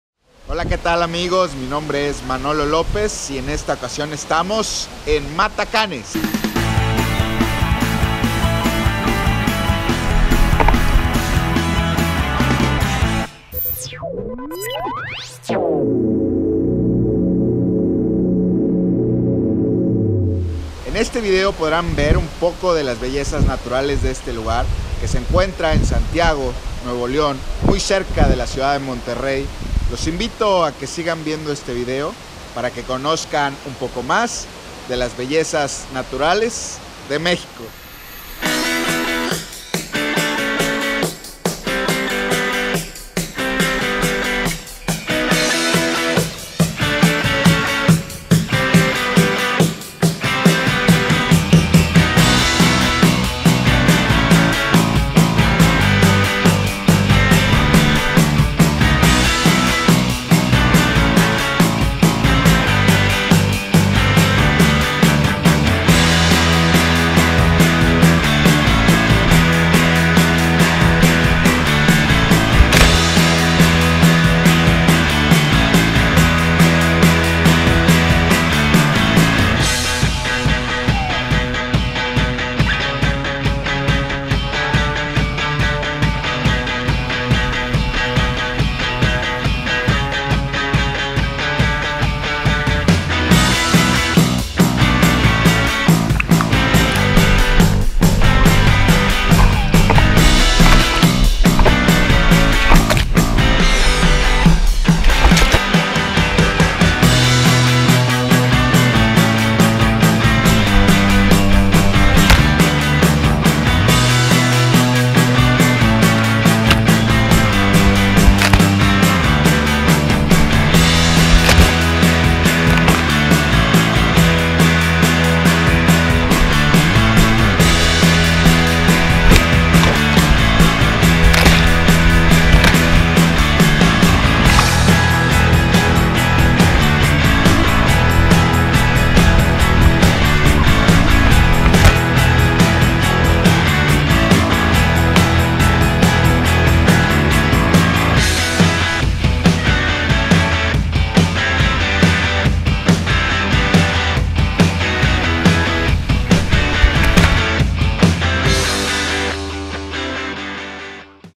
Matacanes N.L. Méx.
Este fin de semana hicimos cañonismo en Matacanes, en Nuevo Leon México, una experiencia increíble que vale la pena vivirla.